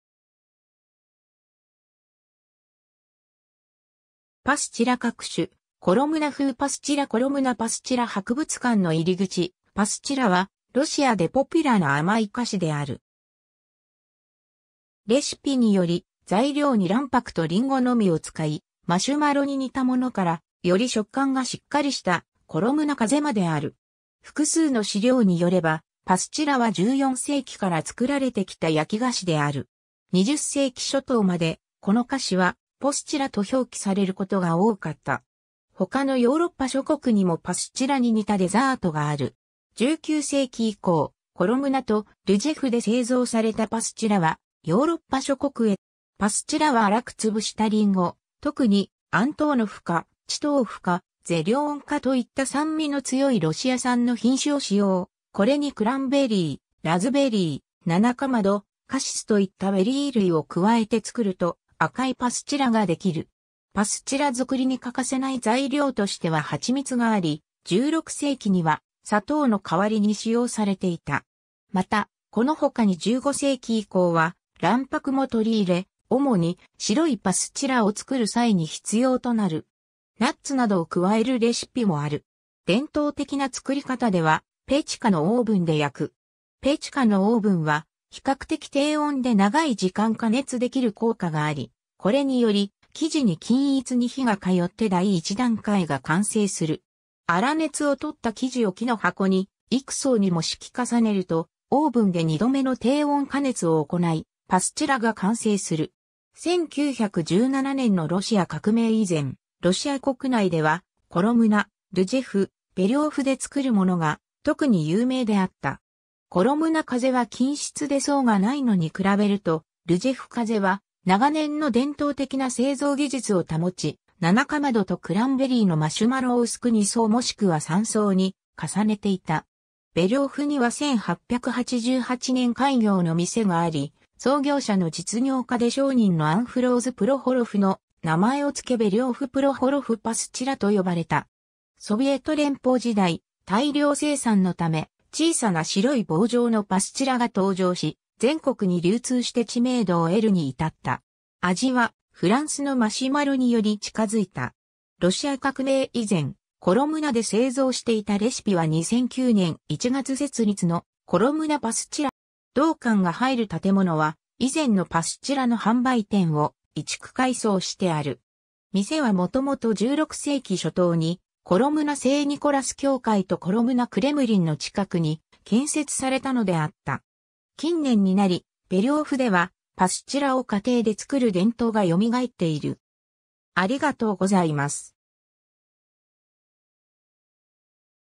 パスチラ
パスチラ各種（コロムナ・パスチラ博物館の試食用） コロムナ風パスチラ コロムナ・パスチラ博物館の入り口 パスチラ（ロシア語: пастила）は、ロシアでポピュラーな甘い菓子である。
レシピにより材料に卵白とリンゴのみを使いマシュマロに似たものから、より食感がしっかりしたコロムナ風まである。
 複数の資料によれば、パスチラは14世紀から作られてきた焼き菓子である（地域誌にはコロムナで開発されたとしている）。
20世紀初頭まで、この菓子はポスチラ（постила）と表記されることが多かった。
他のヨーロッパ諸国にもパスチラに似たデザートがある（例えばフランスのパートドフリュイ）。
19世紀以降、コロムナとルジェフで製造されたパスチラはヨーロッパ諸国へと輸出が始まる。
 パスチラは荒く潰したりんご、特にアントーノフカ、チトーフカ、ゼリョーンカといった酸味の強いロシア産の品種を使用、これにクランベリー、ラズベリー、ナナカマド、カシスといったベリー類を加えて作ると「赤いパスチラ」ができる。
パスチラ作りに欠かせない材料としては蜂蜜があり、16世紀には砂糖の代わりに使用されていた。
また、この他に15世紀以降は卵白も取り入れ、主に「白いパスチラ」を作る際に必要となる。
ナッツなどを加えるレシピもある。
 伝統的な作り方では、ペチカのオーブンで焼く。
ペチカのオーブンは比較的低温で長い時間加熱できる効果があり、これにより生地に均一に火が通って第1段階が完成する。
粗熱を取った生地を木の箱に幾層にも敷き重ねると、オーブンで2度目の低温加熱を行いパスチラが完成する。
 1917年のロシア革命以前、ロシア国内ではコロムナ、ルジェフ、ベリョーフで作るものが特に有名であった。
コロムナ風は均質で層がないのに比べると、ルジェフ風は長年の伝統的な製造技術を保ち、ナナカマドとクランベリーのマシュマロを薄く2層もしくは3層に重ねていた。
ベリョーフには1888年開業の店があり、創業者の実業家で商人のアンフローズ・プロホロフの名前を付けベリョーフ・プロホロフ・パスチラと呼ばれた。
 ソビエト連邦時代、大量生産のため小さな白い棒状のパスチラが登場し、全国に流通して知名度を得るに至った。
味は、フランスのマシュマロにより近づいた。
 ロシア革命以前、コロムナで製造していたレシピは2009年1月設立のコロムナ・パスチラ博物館 で見ることができ、同館ではさまざまなパスチラの試食ができる他、アントーノフカ種のりんごで作るかつてのレシピで作ったものを購入できる。
同館が入る建物は、以前のパスチラの販売店を移築改装してある。
店はもともと16世紀初頭にコロムナ聖ニコラス教会とコロムナ・クレムリンの近くに建設されたのであった。
 近年になりベリョーフでは、パスチラを家庭で作る伝統が蘇っている。
（ベリョーフ・パスチラを参照。）